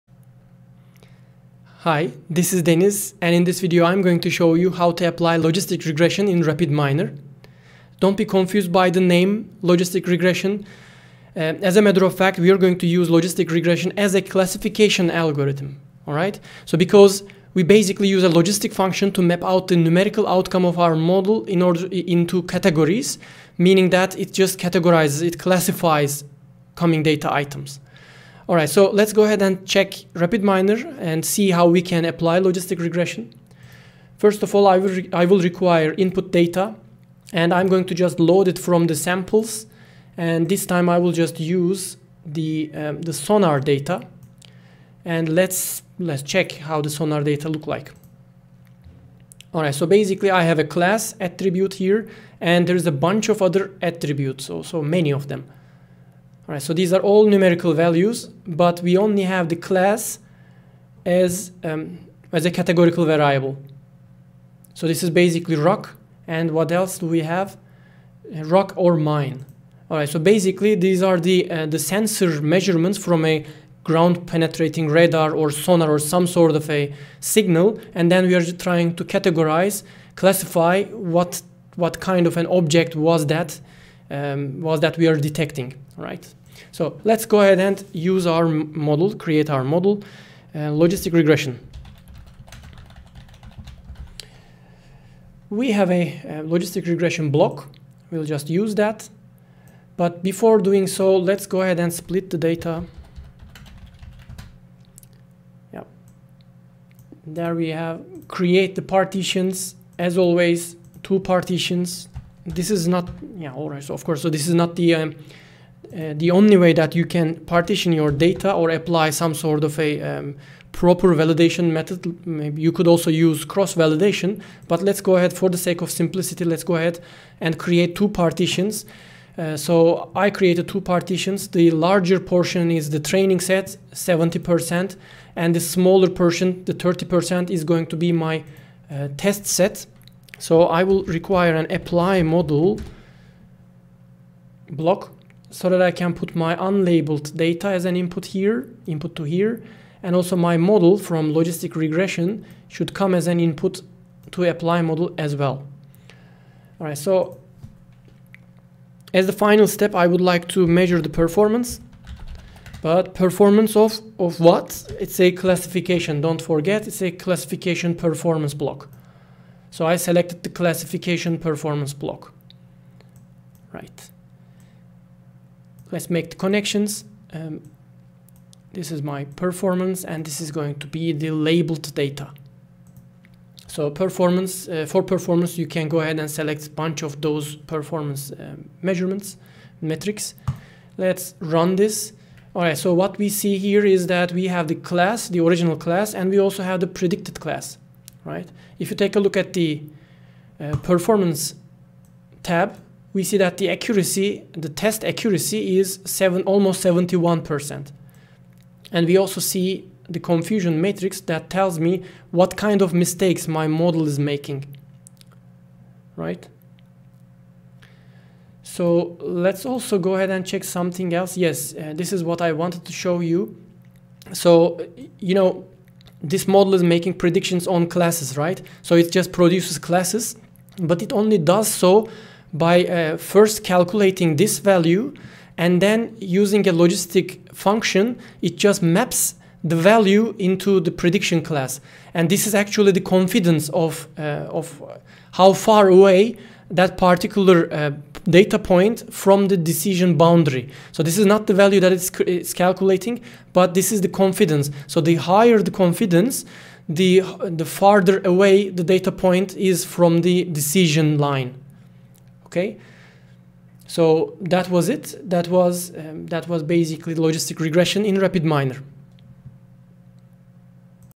Logistic Regression with Rapidminer
In this video, I show how to apply Logistic Regression with Rapidminer.